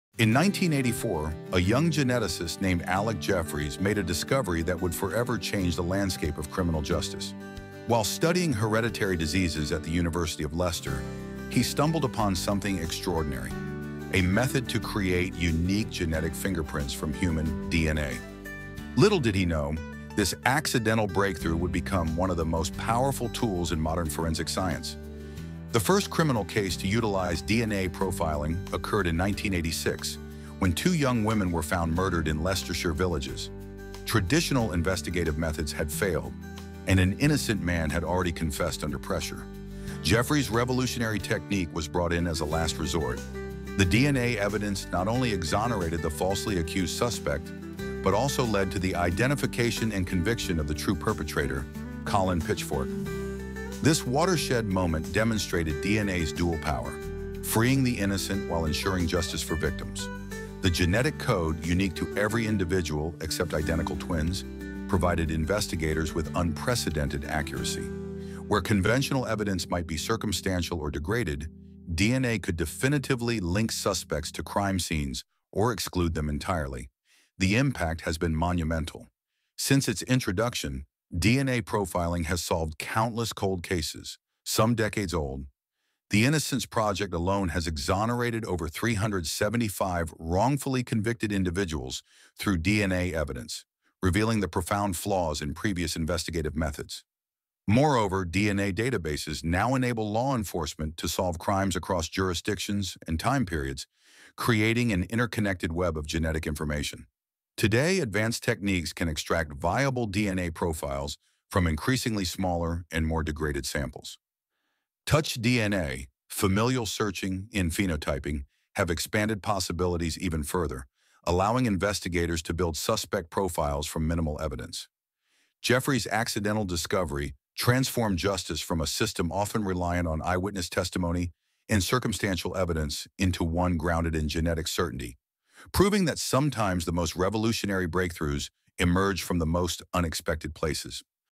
How Forensics is Revolutionizing Justice
In 1984, geneticist Alec Jeffreys accidentally discovered DNA fingerprinting while studying hereditary diseases at the University of Leicester. This groundbreaking technique would revolutionize criminal justice forever.

The first case came in 1986 when two young women were murdered in Leicestershire. Traditional methods had failed, and an innocent man had confessed under pressure. Jeffreys' DNA profiling was used as a last resort, not only exonerating the wrongly accused but also identifying the real killer, Colin Pitchfork.

This watershed moment showcased DNA's incredible dual power: freeing the innocent while ensuring justice for victims. Unlike conventional evidence that can be circumstantial or degraded, genetic codes provide unprecedented accuracy since they're unique to every individual except identical twins.

The impact has been transformative. DNA profiling has solved countless cold cases spanning decades. The Innocence Project alone has exonerated over 375 wrongfully convicted people through DNA evidence, exposing serious flaws in previous investigative methods.

Today's advanced techniques can extract DNA from increasingly smaller samples. Touch DNA, familial searching, and phenotyping allow investigators to build suspect profiles from minimal evidence. What began as an accidental laboratory discovery has transformed our justice system from one relying on eyewitness testimony to one grounded in genetic certainty.